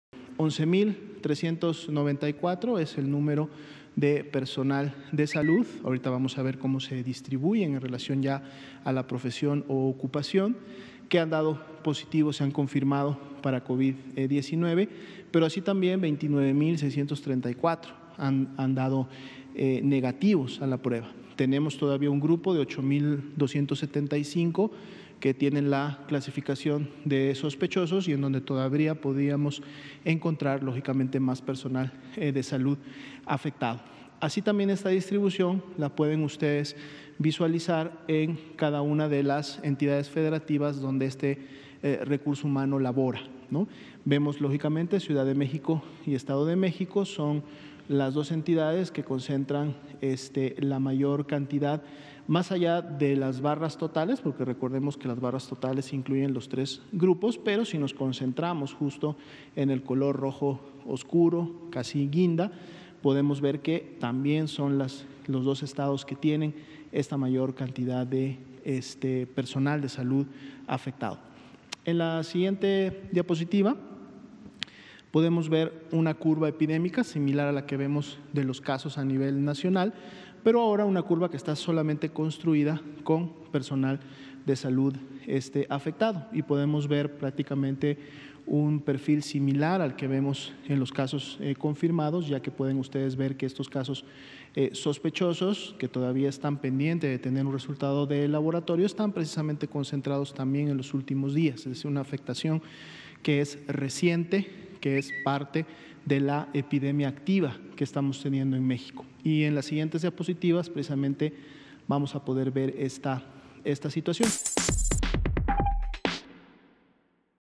11 mil 394 casos positivos de COVID-19 en personal de Salud
El director de Epidemiología, José Luis Alomía, informó que 11 mil 394 personas del equipo de salud en México han dado positivo para coronavirus.